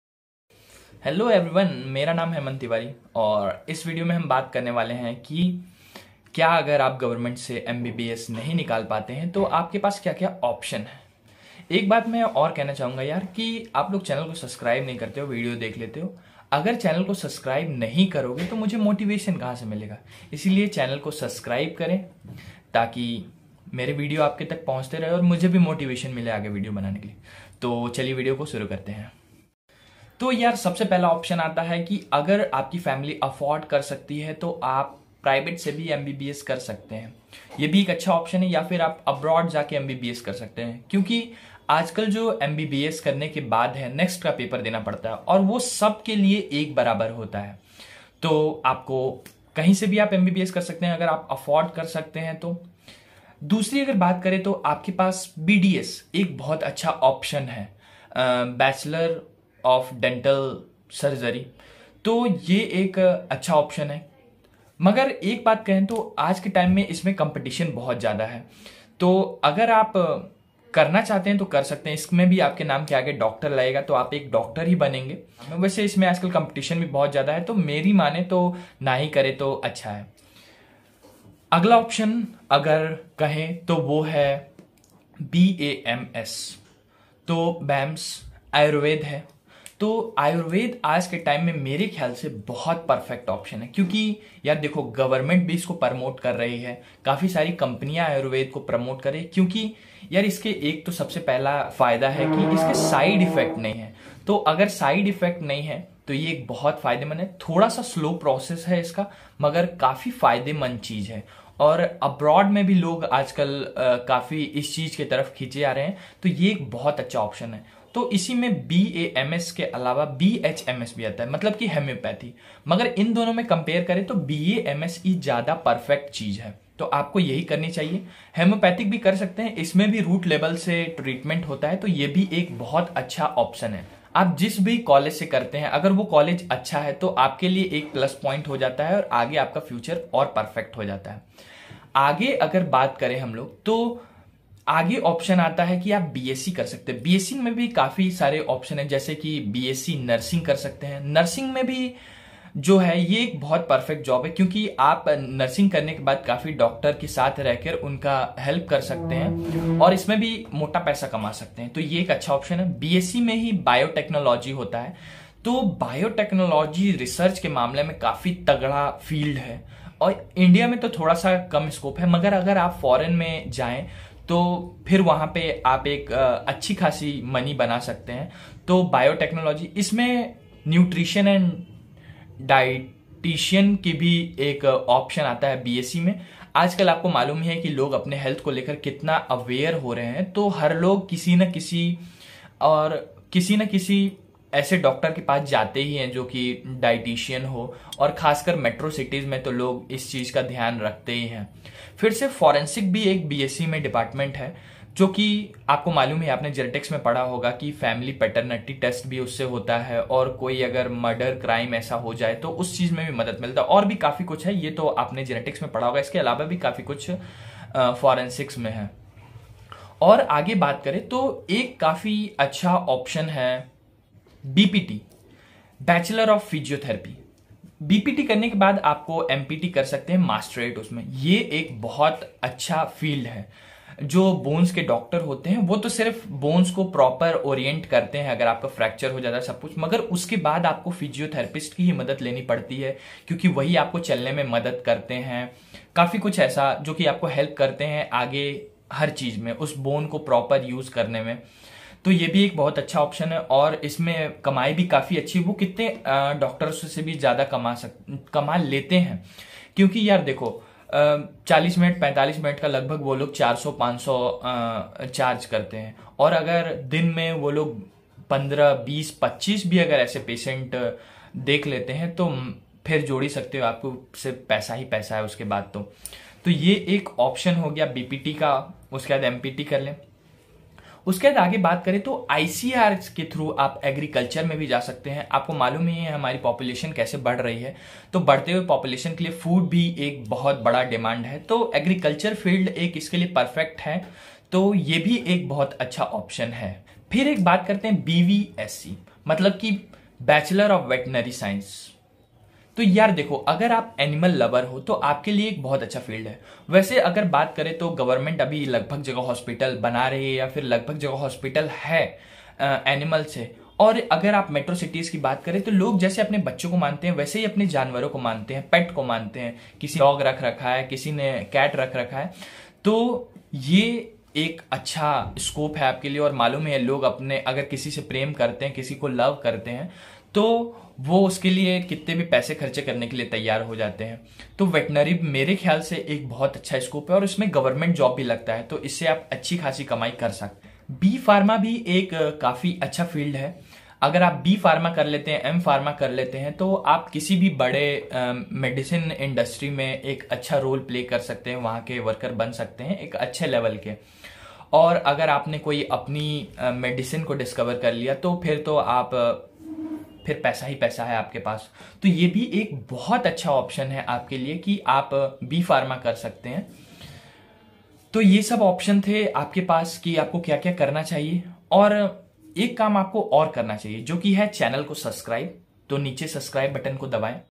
Getting 300,400,500 in NEET2022||what are the options?||NEET 2022||Aman Tiwari
In this video I am talking about all other options than government mbbs